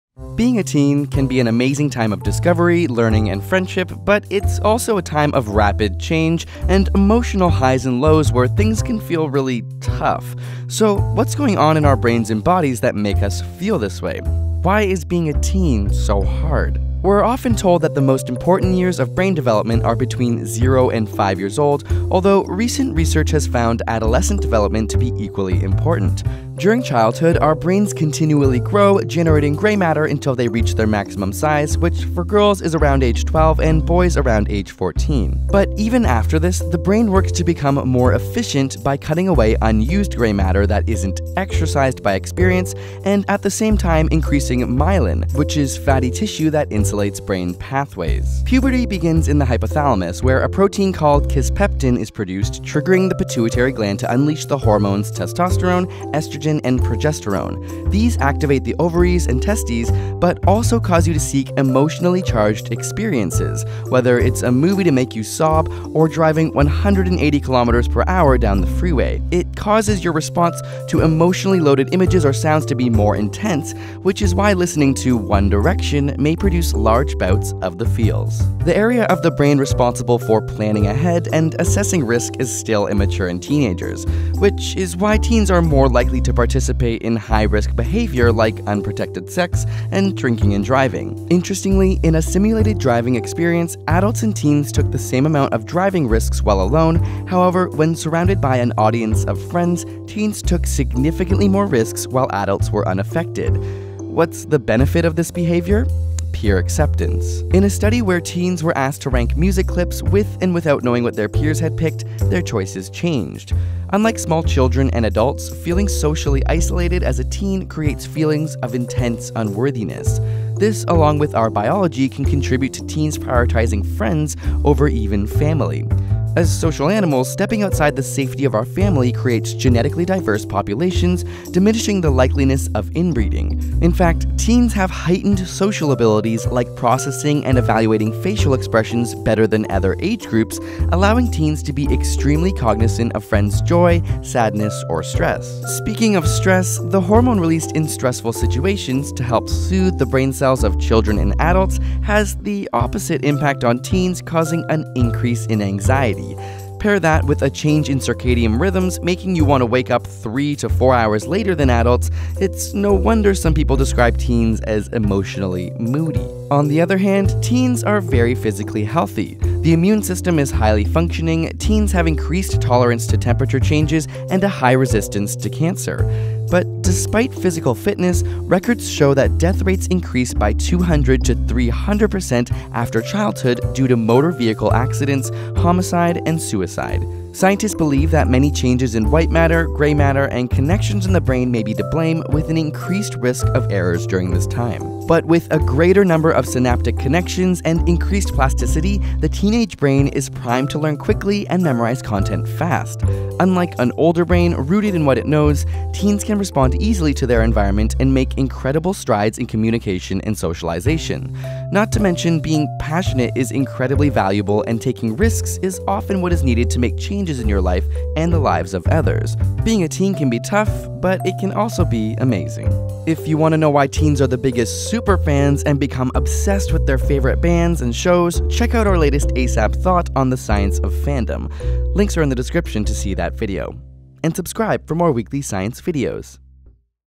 Why Are Teens So Moody?
A look inside the teenage brain!

Written by Rachel Salt, Mitchell Moffit and Gregory Brown

ASAPSCIENCE INC.

Further Reading:

References
Brain development in children and adolescents: insights from anatomical magnetic resonance imaging.

GABA receptors make teens resistant to input

The Teenage Brain - A neuroscientist’s survival guide to raising adolescents and young adults.  Frances E. Jensen and Amy Ellis Nutt
Sleep variability in adolescence is associated with altered brain development
Is Adolescence a Sensitive Period for Sociocultural Processing?
Inside your teenager’s scary brain

Risk and Rationality in Adolescent Decision Making